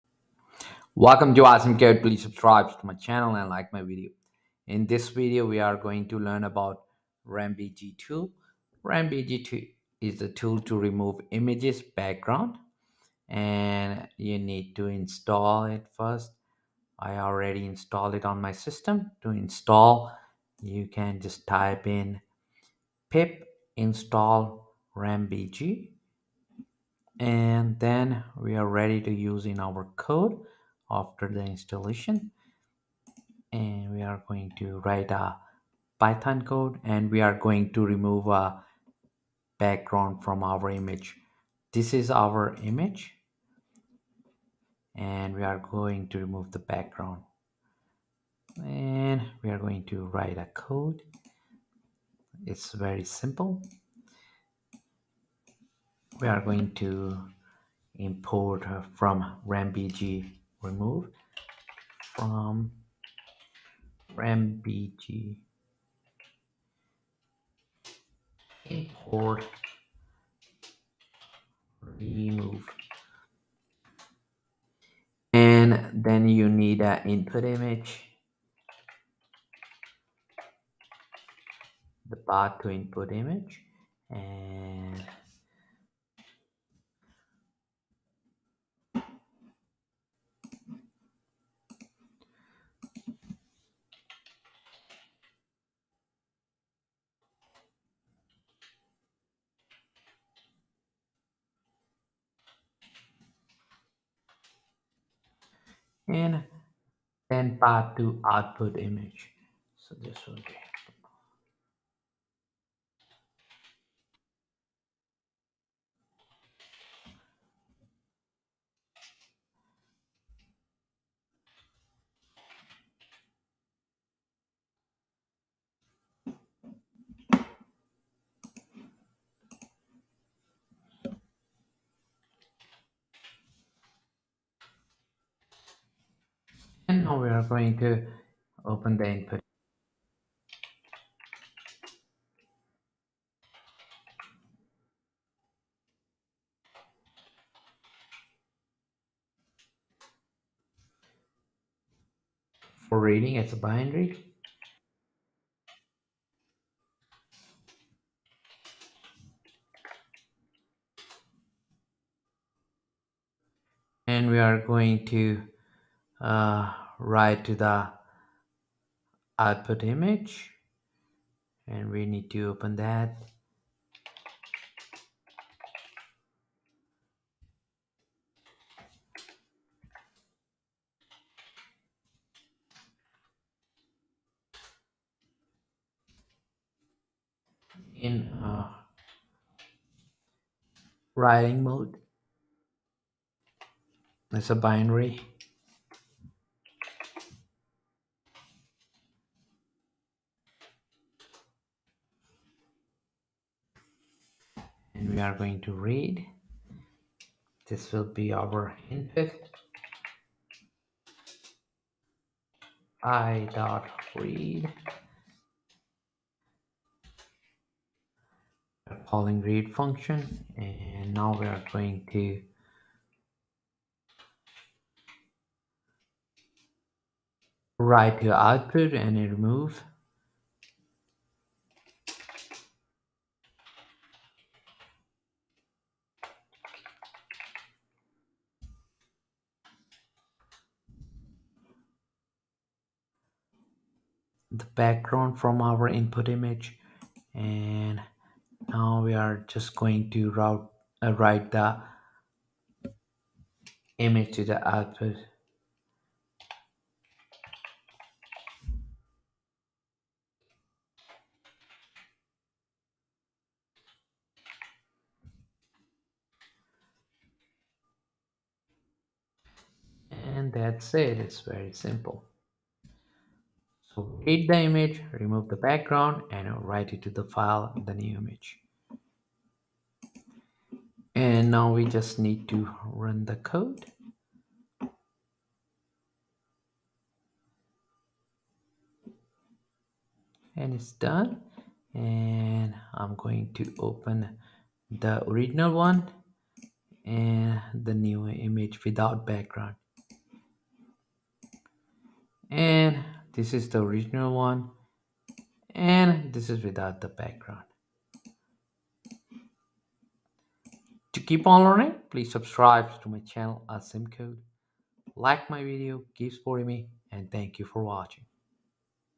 How to remove images background with Rembg using Python
Rembg is a tool to remove images background.  Code